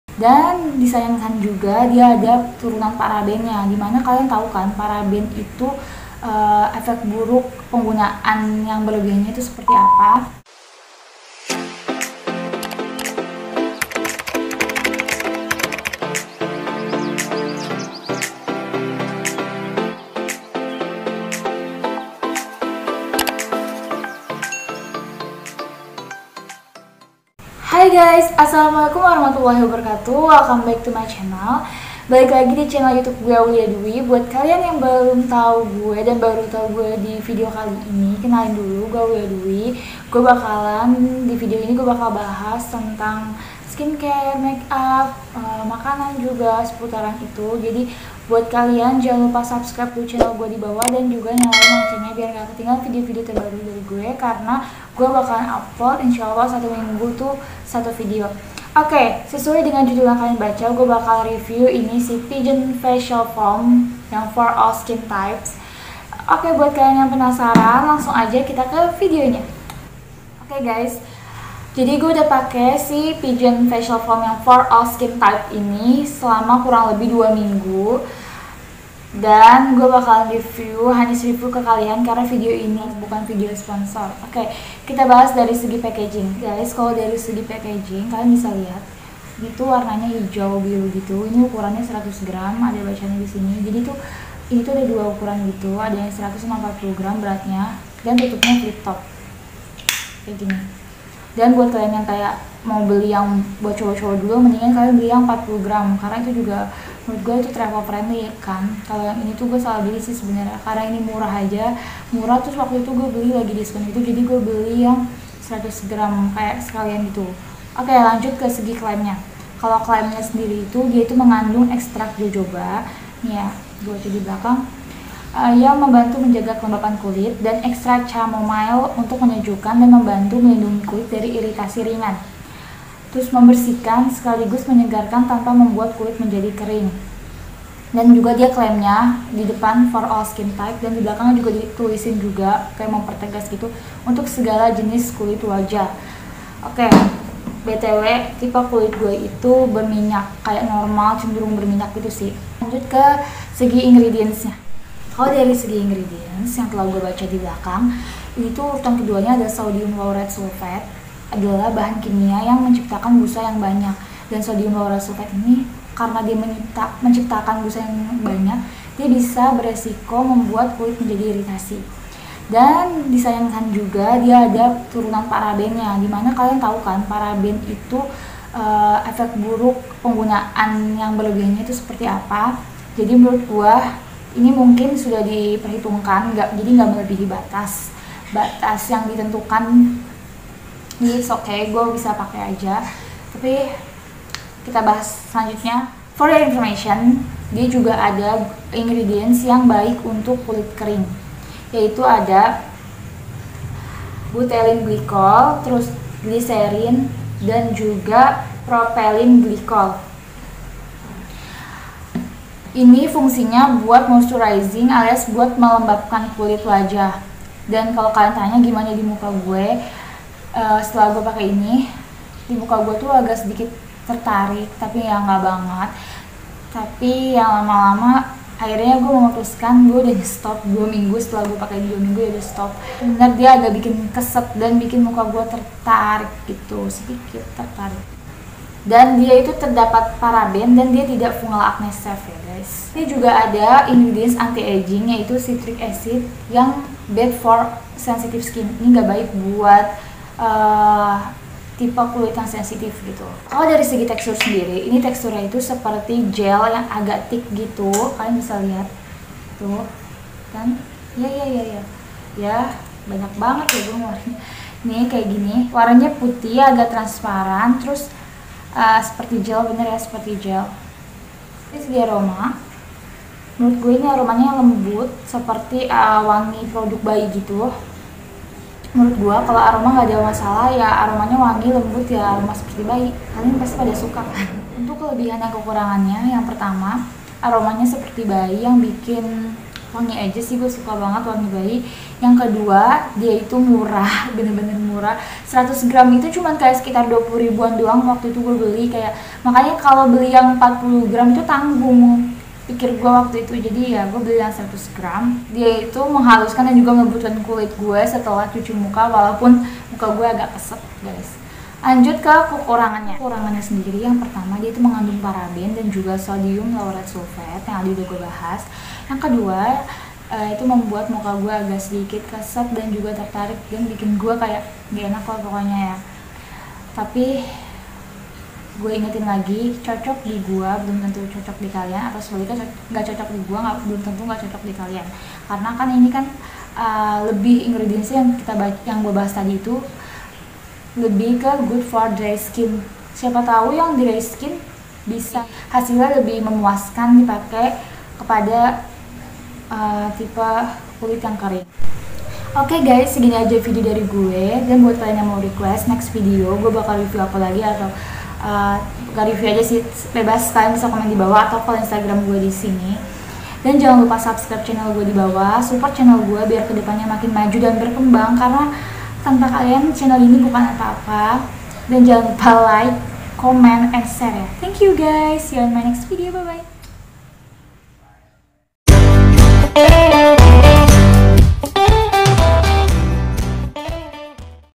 Review Pigeon Facial Foam, TERNYATA ADA PARABEN NYA?
Hallo guys jadi disini aku Review Pigeon Facial Foam
Apakah benar ada Paraben nya?
Yuk nonton videonya sampe habis ya guys!

•Thank you guys, HAPPY WATCHING!